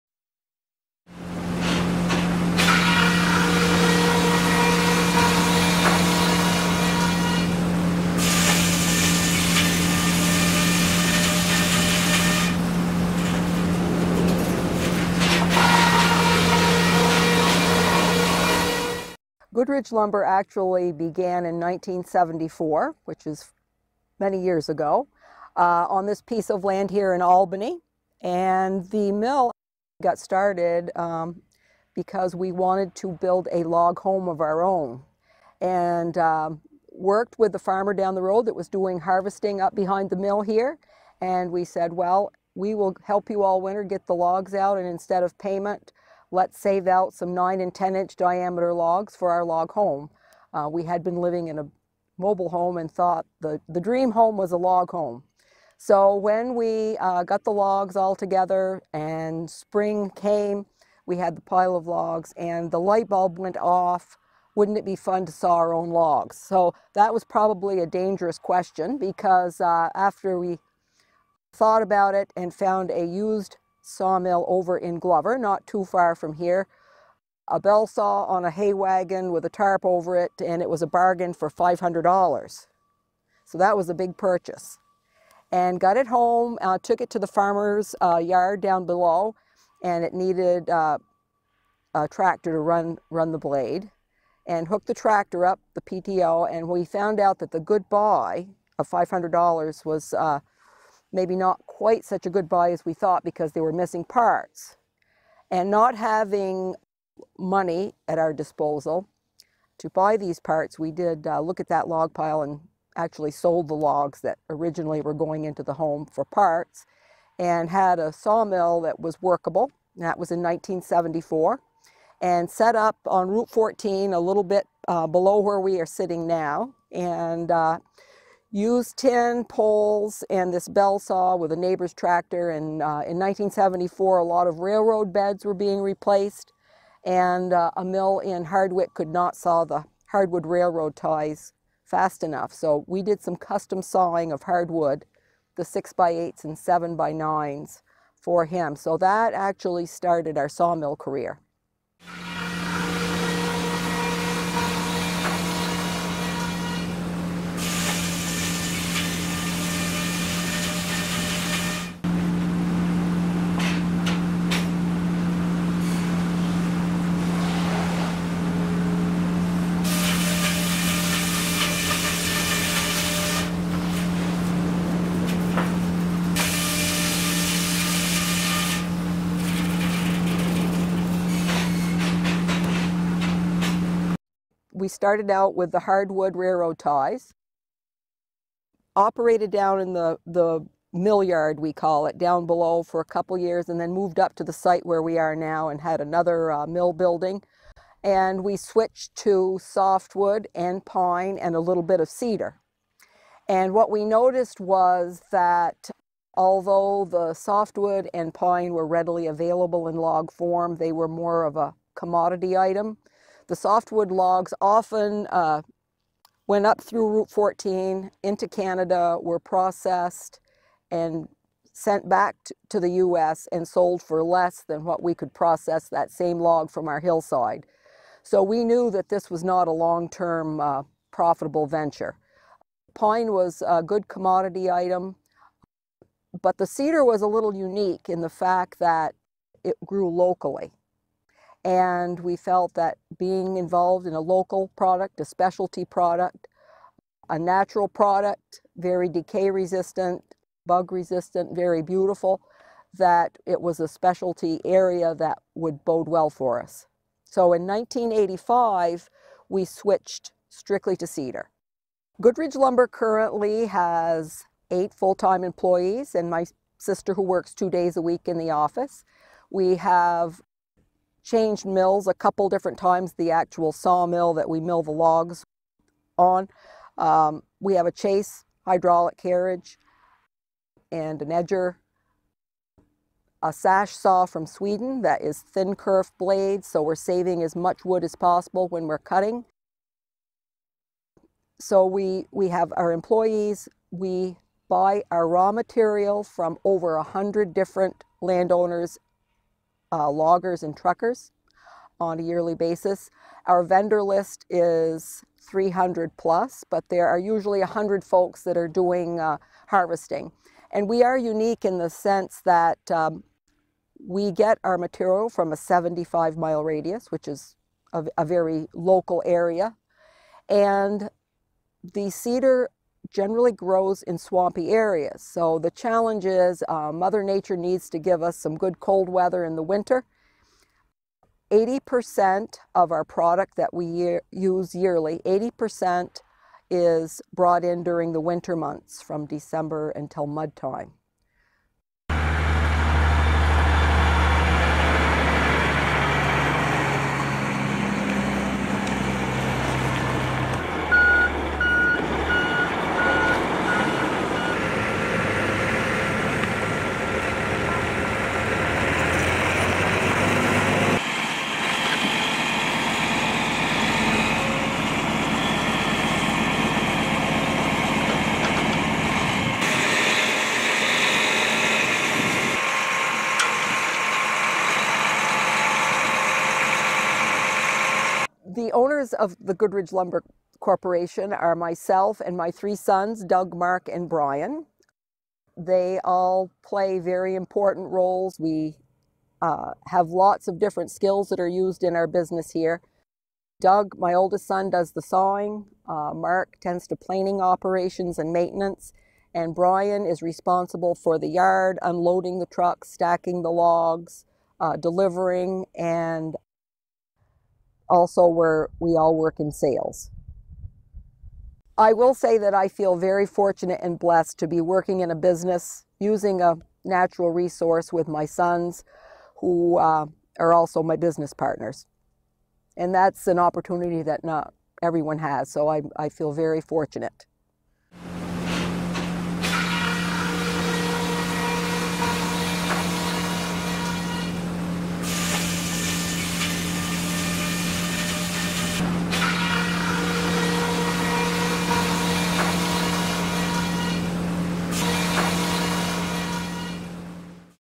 Goodridge Lumber Company, Albany,VT Profile
A Vermont Rural Careers Project Profile to encourage urban and suburban residents  to consider  careers in the working landscape industries. This Profile was funded by the Plum Creek Timber Company's Foundation  and produced by Farm & Forest Productions, Cabot, VT,  in 2016.